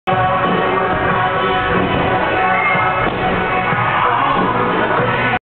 Kristin Milardo's Full-in on Floor
Kristin trains her full-in on floor at Colorado Aerials.